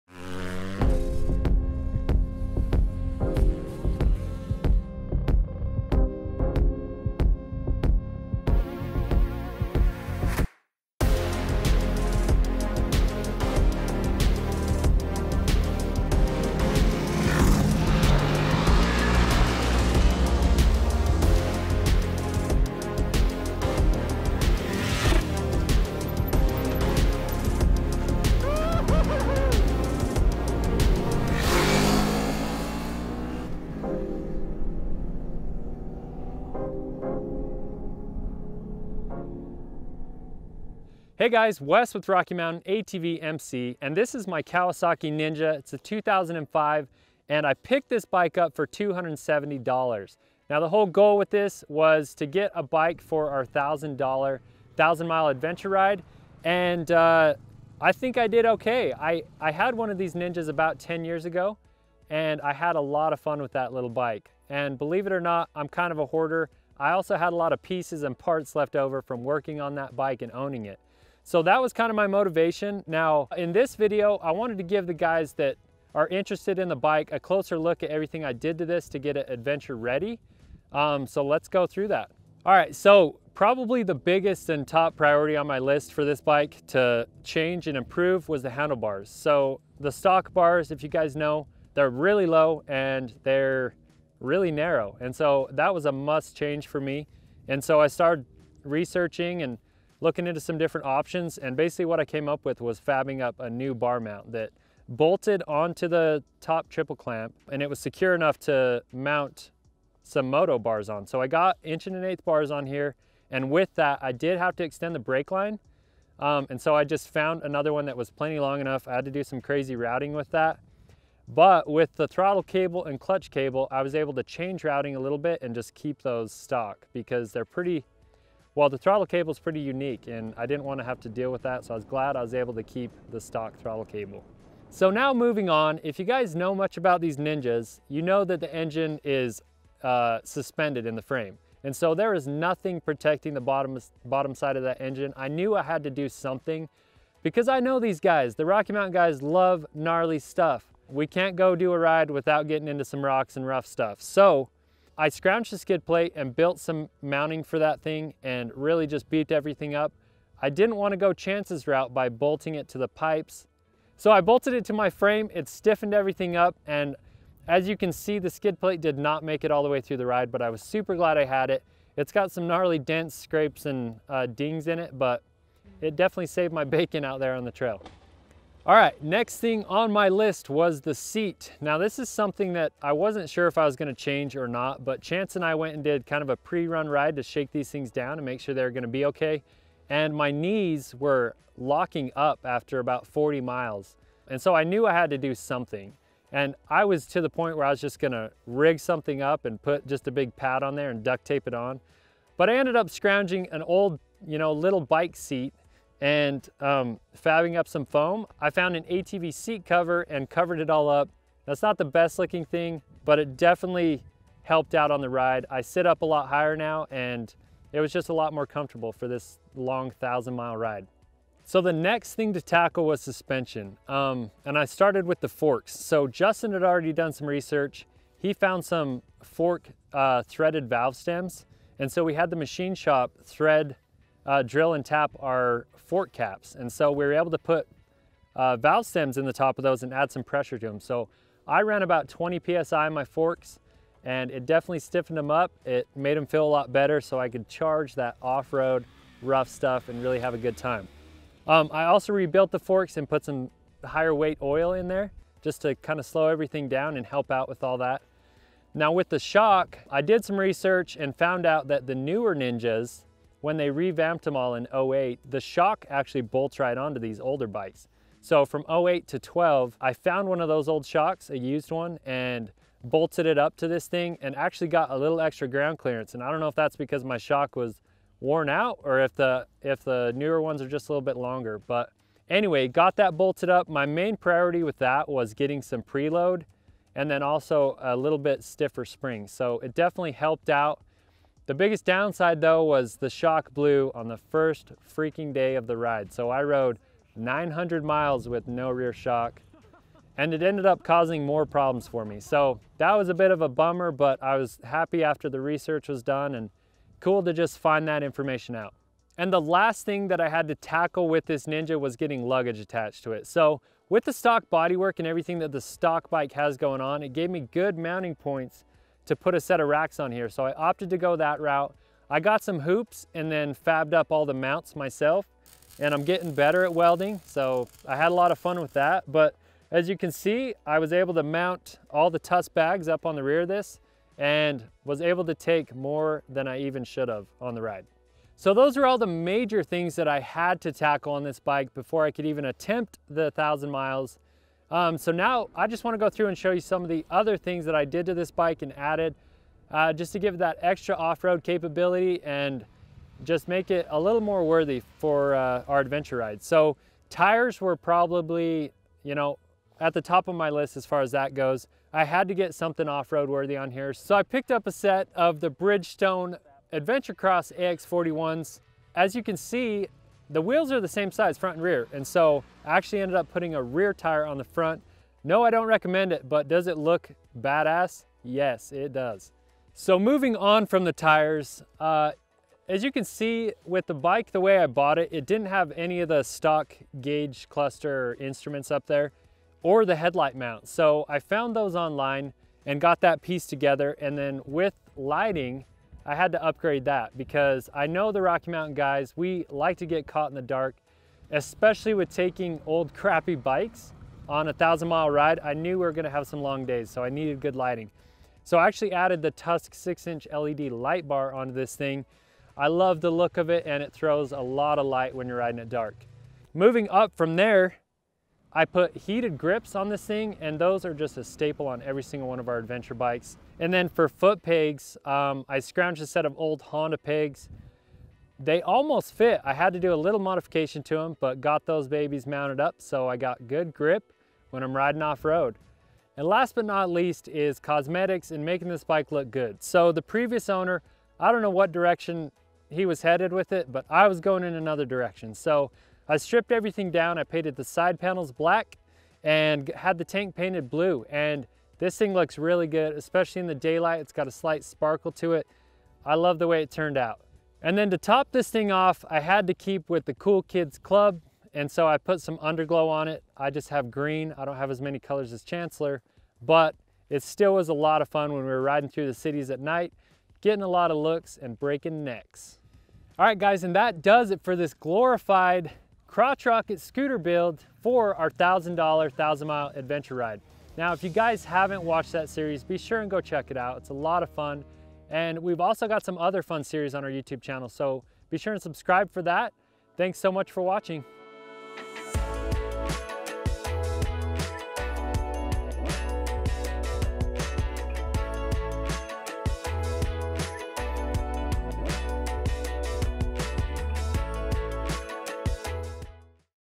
2005 Kawasaki Ninja 250 Adventure Bike Build
Wes picked up this 2005 Kawasaki Ninja 250 for $270 to take on our $1000 Motorcycles vs 1,000 ADV Miles ride series. We all know that the Ninja 250 was never designed to be taken off-road, but there are a few parts and modifications that could be made to make the dirt a slightly more enjoyable ride. Take a closer look at what it took to get this street bike ready to tackle 1,000 adventure miles as Wes gives a rundown on his 2005 Kawasaki Ninja 250 adventure bike build!

for all your parts, apparel, and accessories!